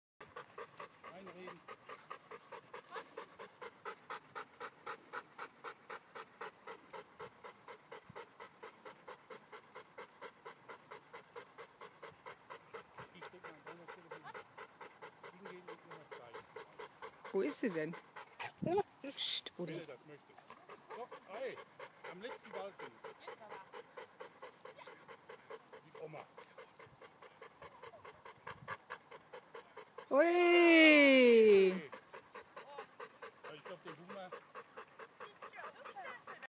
Training 24.08.13
Caya beim Training am 28.08.13! Wer hat mehr Spaß? Hund oder Trainer?
Danke Reggi fürs filmen!